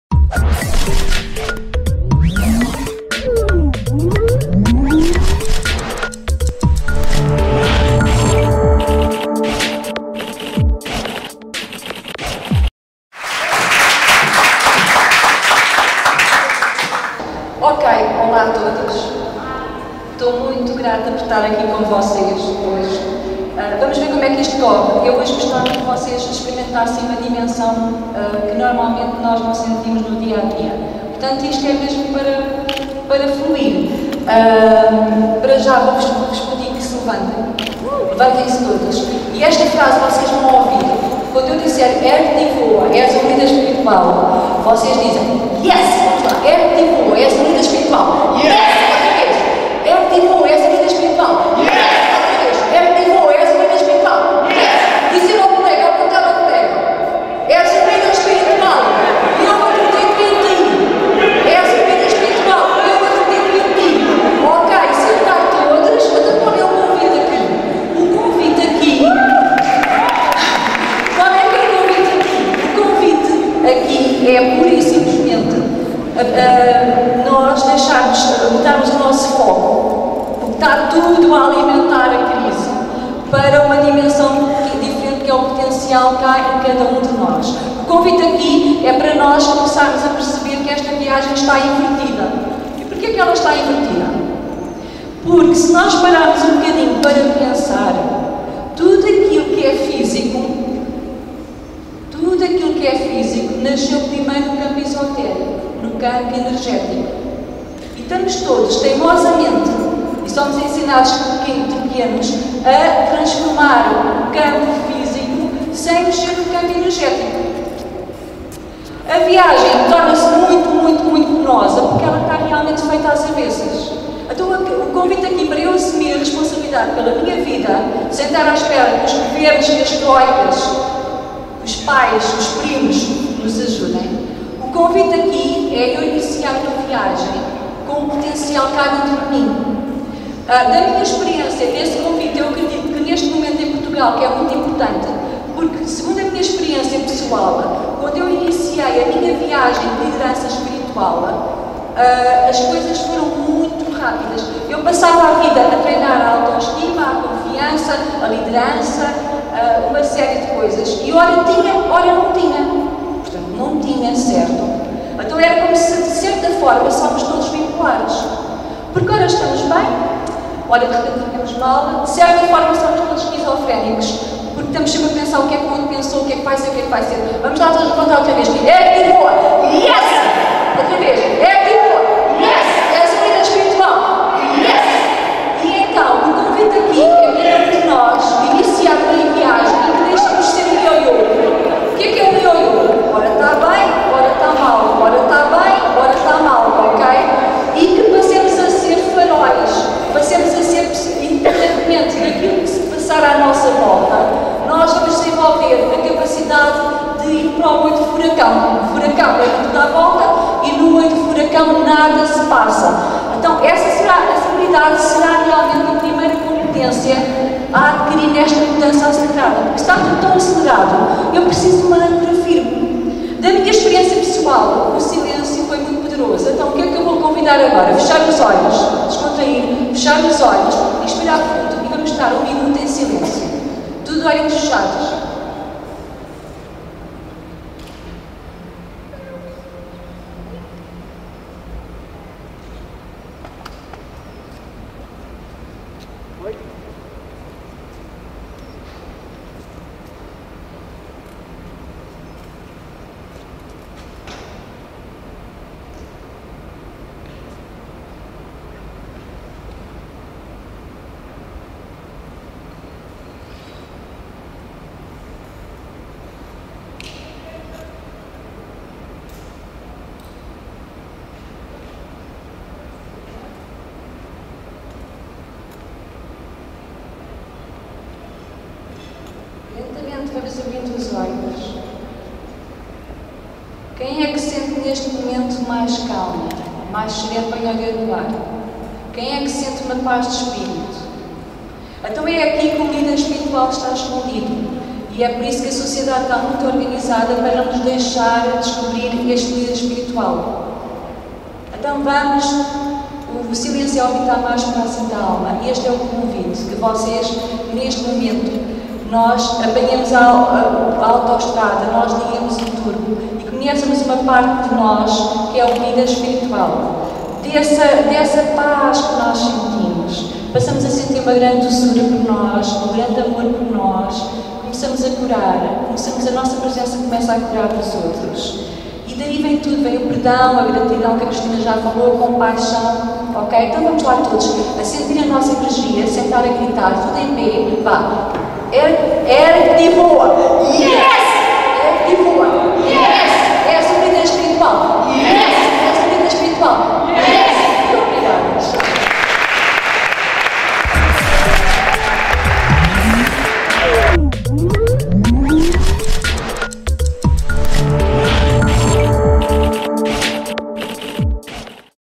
[Ignite #37] Kyra Abreu - Ergue-te e VOA - Resgate da LIDERANÇA ESPIRITUAL
Em tempos desafiantes precisamos de algo maior que torne o caminho mais suave: acender o Líder/Mestre Espiritual que há em cada um de nós.

Este é o convite, a provocação. Tomar consciência de que tudo o que existe no plano físico (incluindo nós) se iniciou no plano energético. Temos estado a fazer a viagem inversa: tentar alterar  o plano físico sem esta percepção.

"Ergue-te e Voa" deixou toda a gente a pensar e a sentir a força da sua própria energia.

Filmagem: Mário Pires
Edição: Active Media